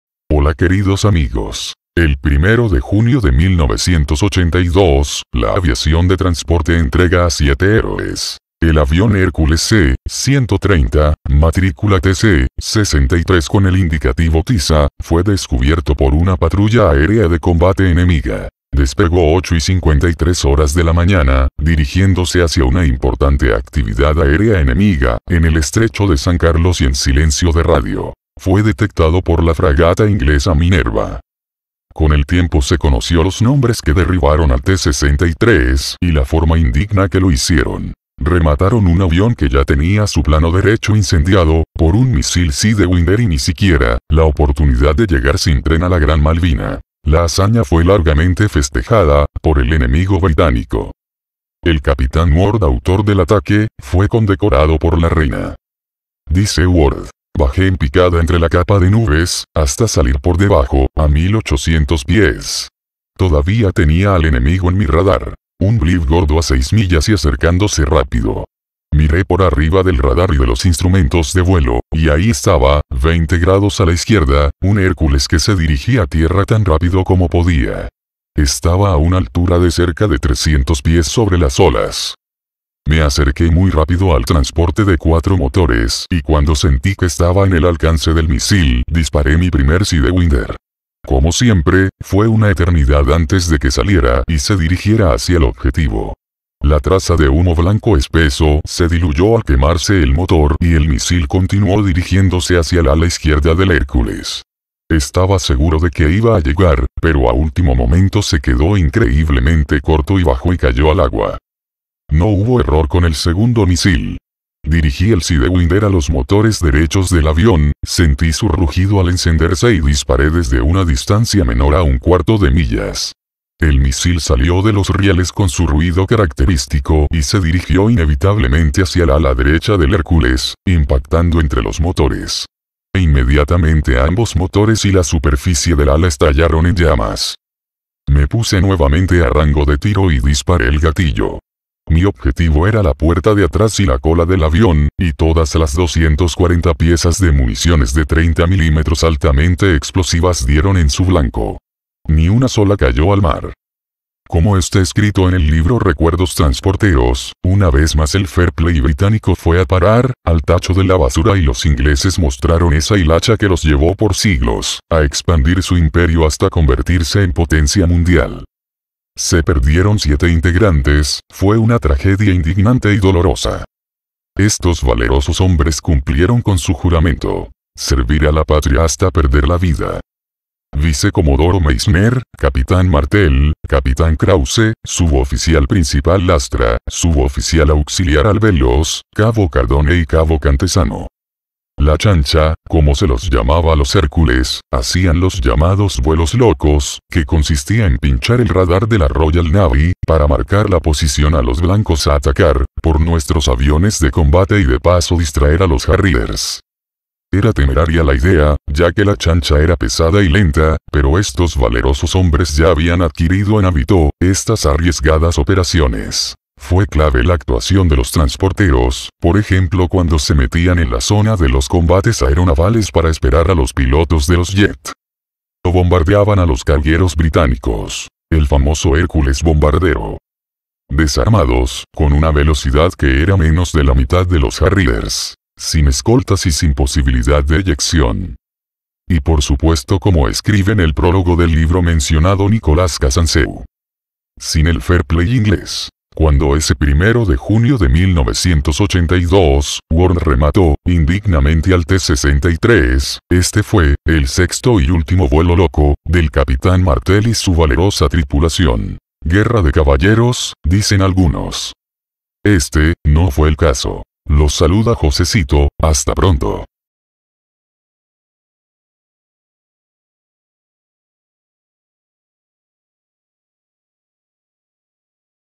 un HARRIER REMATÓ un AVION ARGENTINO EN LLAMAS Y DESARMADO
El 1 de junio de 1982 un trasportero argentino fue rematado por britanicos cayendo el avion con sus tripulantes y desintegrandose en el agua.
LEER EL LIBRO  MALVINAS 1982 RECUERDO TRANSPORTEROS DE  VGM ALFREDO CANO.